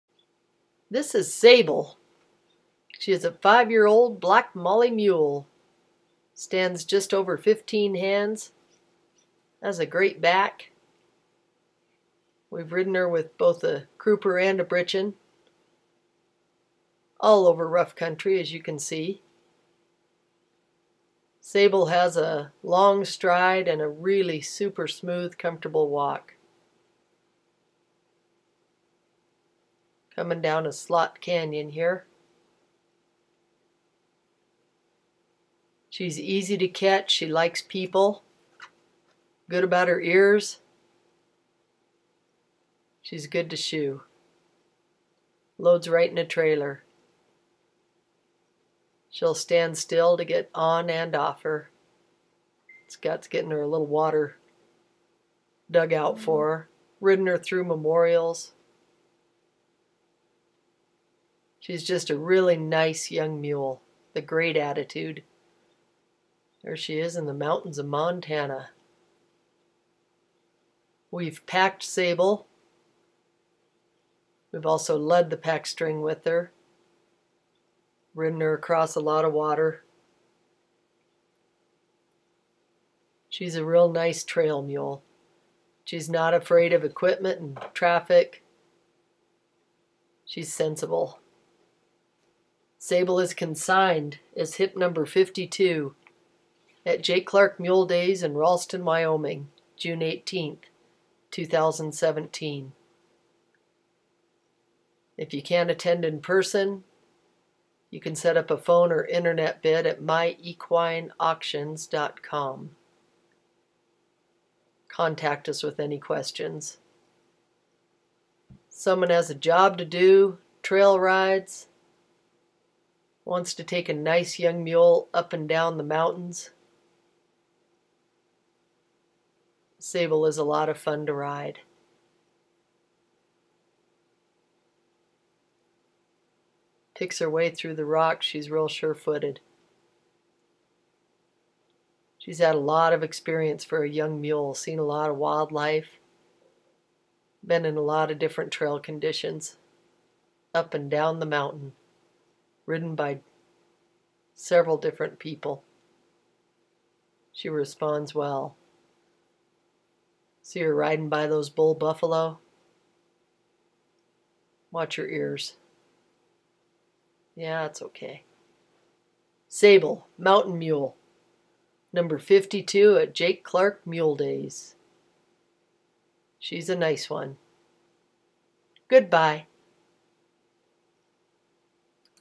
HIP #52, SABLE, consigned by Scott and Sandy Sallee to Jake Clark Mule Days June 18, 2017
SABLE is an easy-going five year old black molly mule.  She stands just over 15 hands.  She is easy to catch and likes people.  Sable stands to get on and off her.  She has led the pack string and been packed.  She has a super-comfortable long smooth stride that really can cover the country.  Sable is a willing mule who has crossed bridges, deadfall, and rivers.  She has seen a lot of wildlife and different country.  She has been ridden by several levels of riders.  She is used to four-wheelers and dogs, being ridden alone and in company, both leading and following.  Sable likes to travel and is a lot of fun to ride.  She is accustomed to either a britching or a crupper.  She is easy to shoe and good to load.  She is a smart young trail and mountain molly that wants to please.  We have ridden her up and down the mountains and through the rivers of Montana, and in the slot canyons and red rocks of Utah.  Sable is consigned as Hip #52 to Jake Clark Mule Days in Ralston, Wyoming June 18, 2017.  If you cannot attend the sale in person you can line up a phone or internet bid